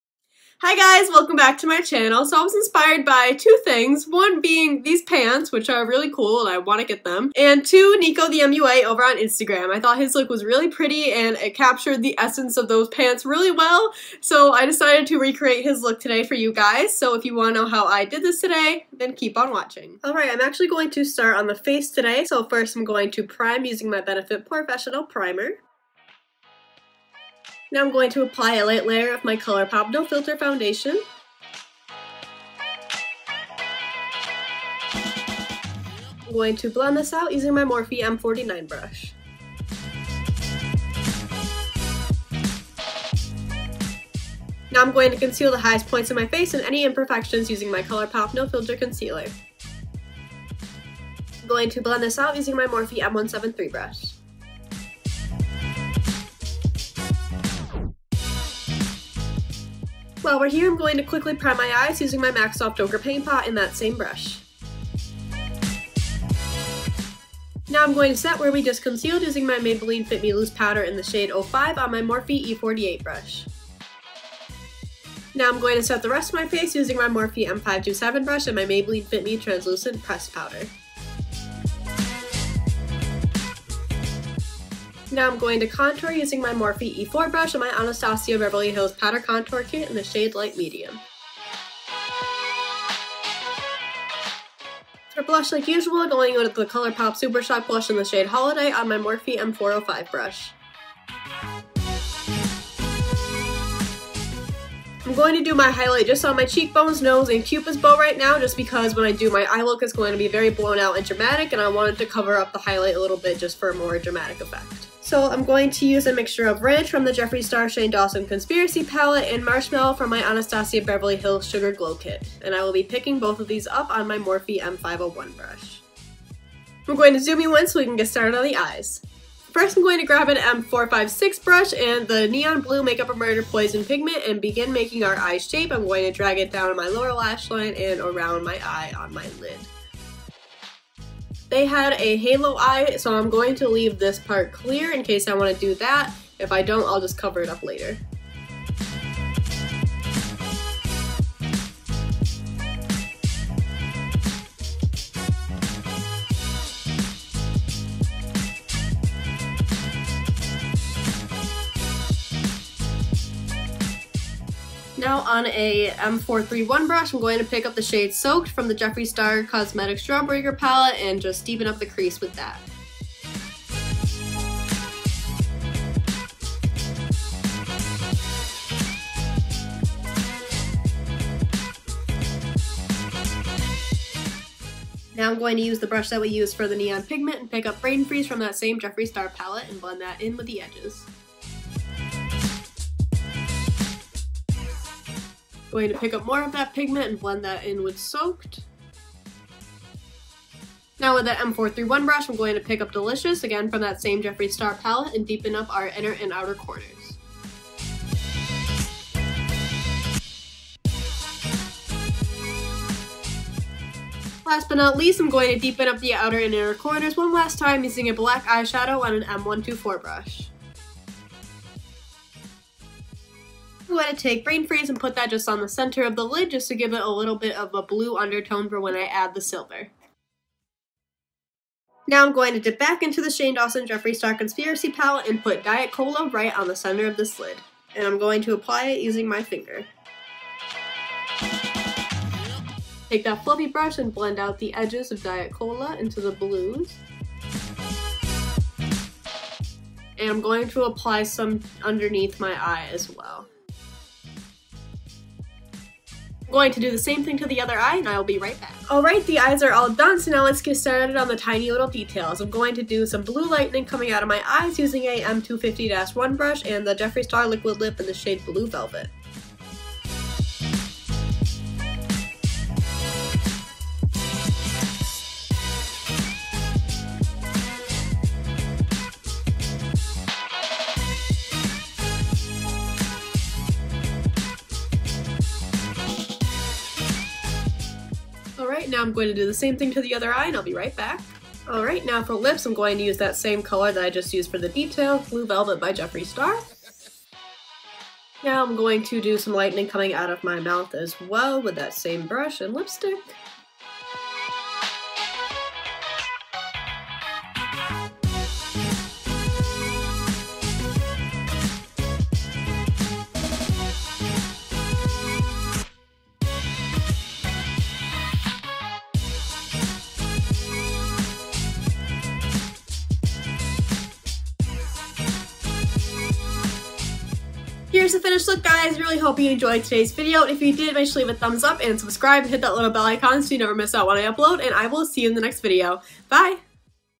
Blue Lightning Halo Eye Tutorial
Hi guys, welcome back to my channel! today i was inspired by @nicothemua on instagram along with some super stellar blue lightning pants! i hope you love the outcome as much as i do and don't forget to subscribe!!